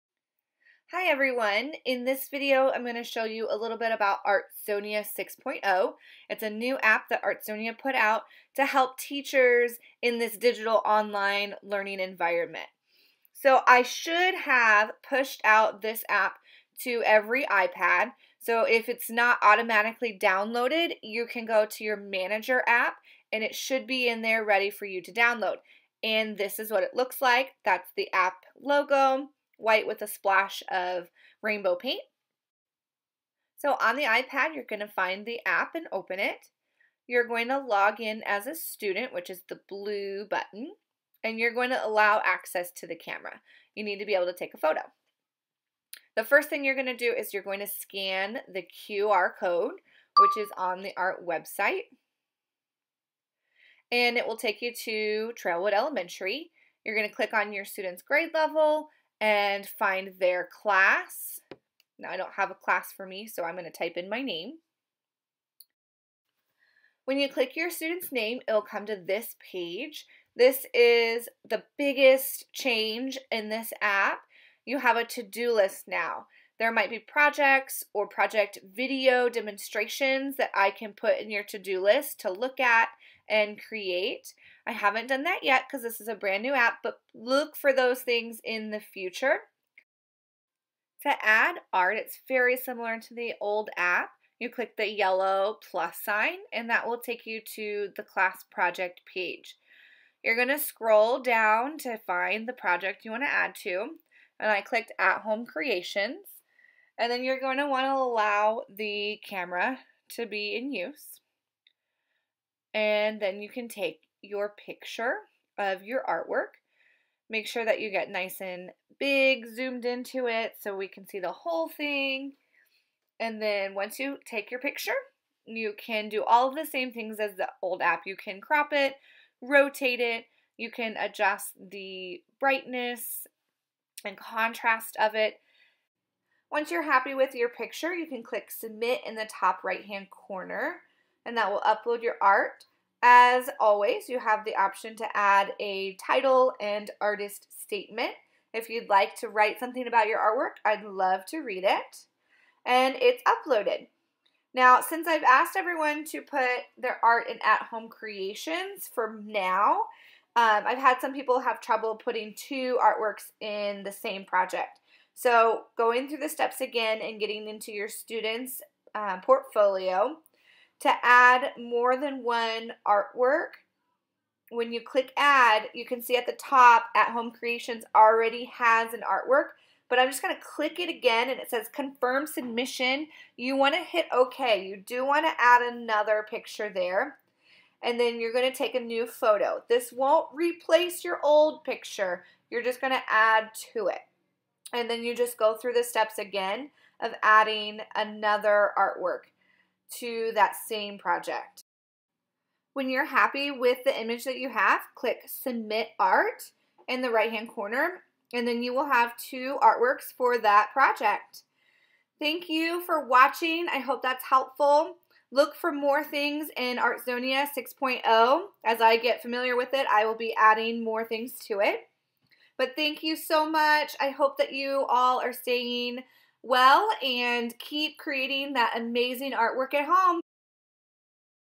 Artsonia 6.0
How to add art in Artsonia 6.0 App